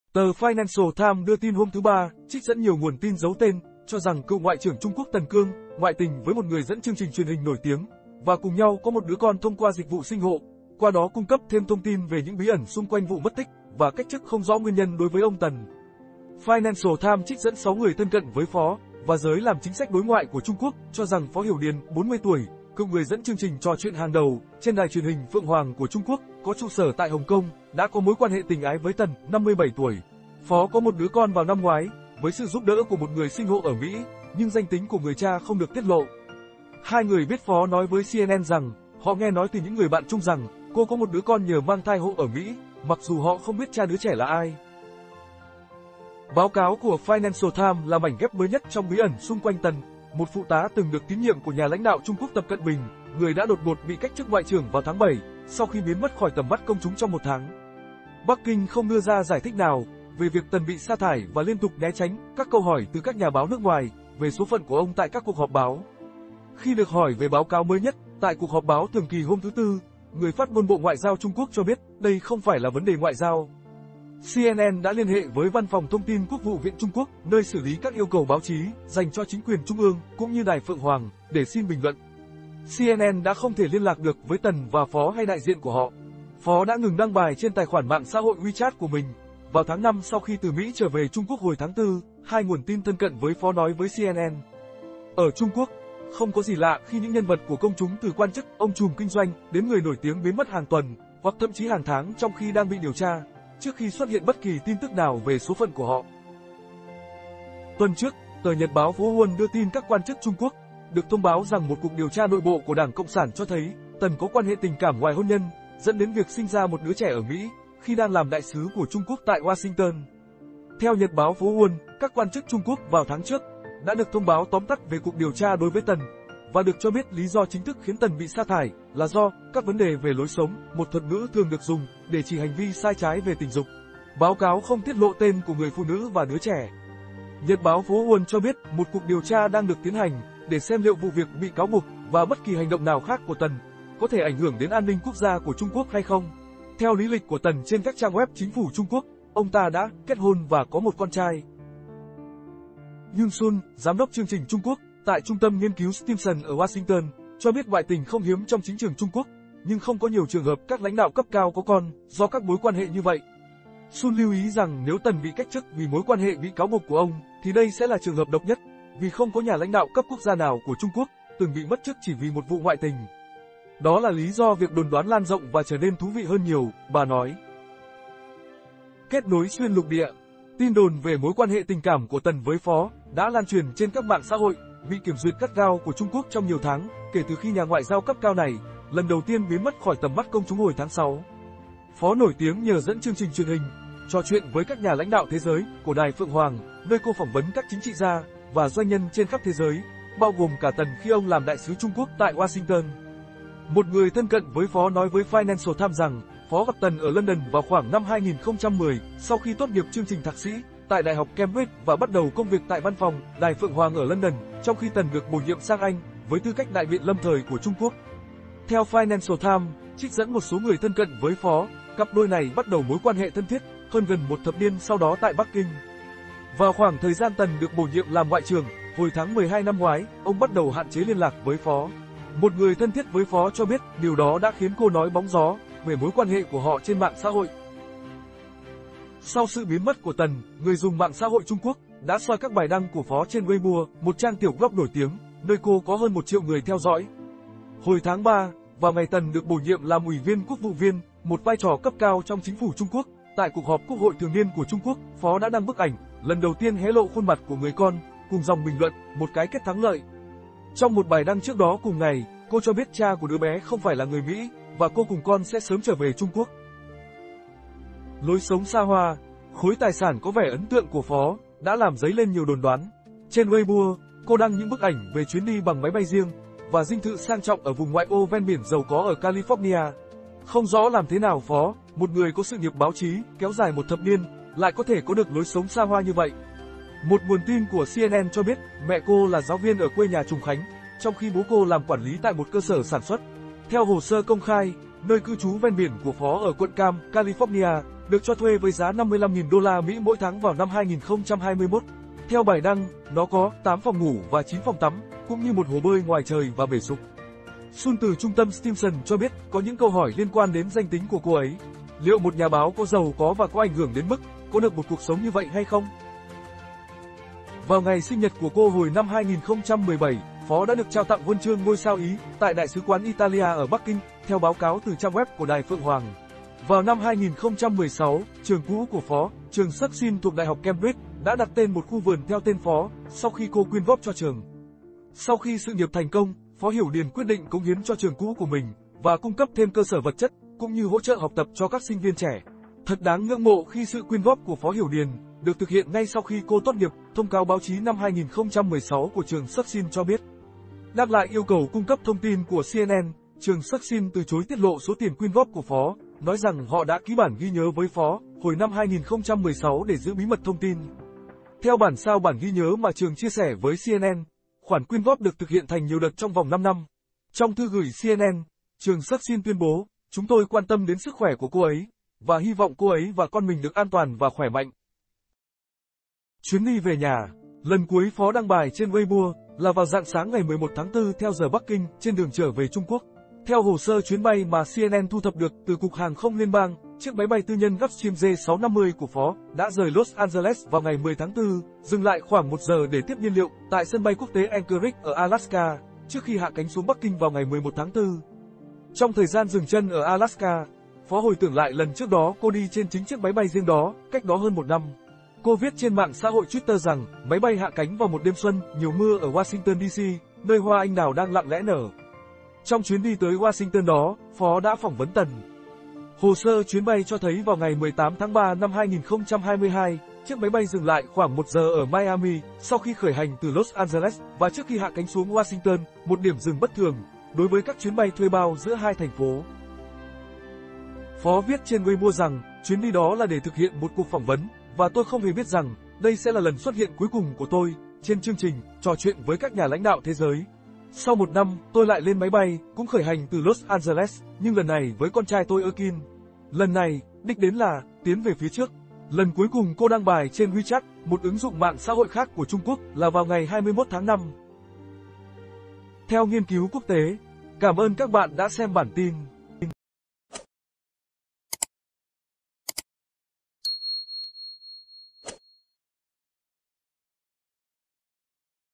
PHÓ HIỂU ĐIỀN - CÓ PHẢI LÀ NGUYÊN NHÂN THẬT SỰ TRONG VIỆC NGOẠI TRƯỞNG TẦN CƯƠNG MẤT CHỨC?
Tờ Financial Times đưa tin hôm thứ Ba, trích dẫn nhiều nguồn tin giấu tên, cho rằng cựu Ngoại trưởng Trung Quốc Tần Cương, ngoại tình với một người dẫn chương trình truyền hình nổi tiếng, và cùng nhau có một đứa con thông qua dịch vụ sinh hộ, qua đó cung cấp thêm thông tin về những bí ẩn xung quanh vụ mất tích, và cách chức không rõ nguyên nhân đối với ông Tần.